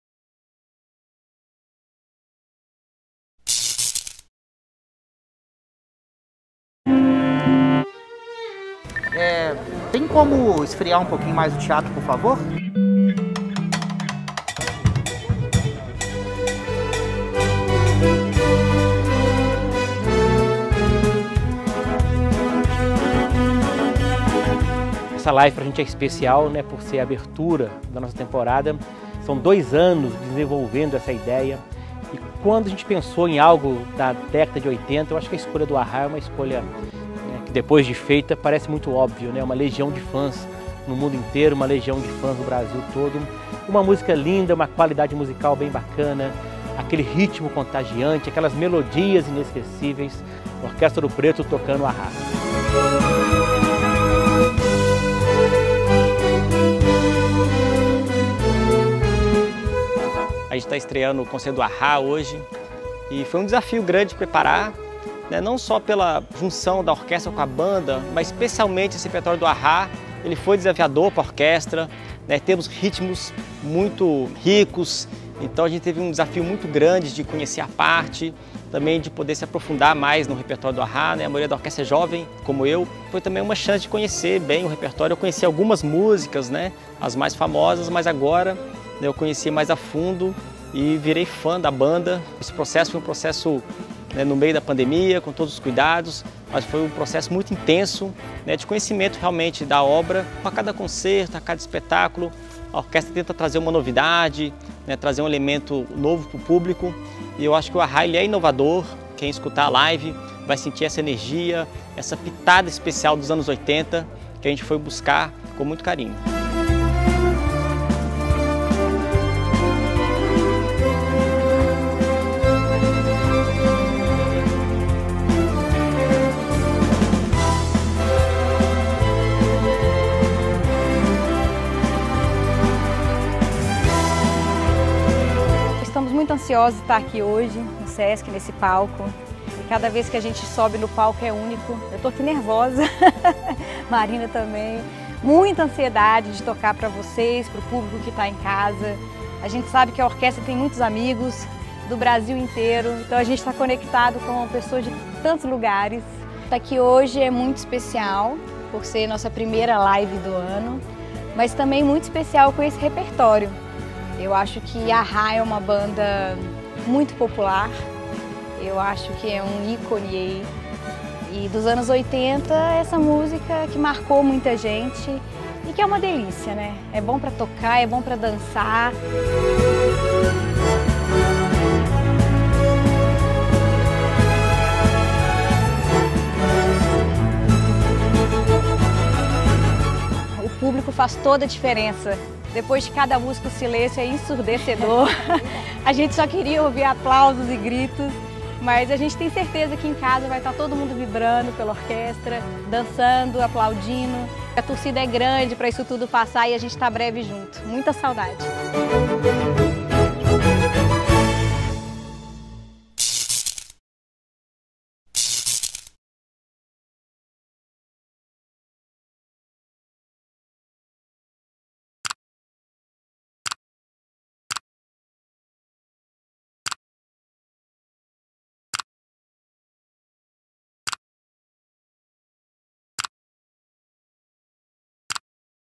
Making Of - Orquestra Ouro Preto - A-Ha
O cuidado em cada detalhe para que a música possa emocionar o público em casa é o que move toda a equipe que está por trás das câmeras. A abertura da Temporada 2021 da Orquestra Ouro Preto, no dia 1º de maio, em homenagem à banda A-Ha foi marcada por emoção, nostalgia, mas também por muita dedicação e cuidados especiais com a saúde e segurança de todos e todas.

Instrumentos afinados, luzes na medida certa, partituras organizadas, álcool em gel em tudo e diversos outros detalhes que, resultaram em uma live que é sucesso de números, com mais de 100 mil visualizações. Confira aqui o making of desse espetáculo.

E para quem ainda não viu ou quer curtir de novo, a apresentação ainda está disponível nosso canal no YouTube.

Apoio: Sesc/MG. Patrocínio: Instituto Cultural Vale, através da Lei Federal de Incentivo a Cultura.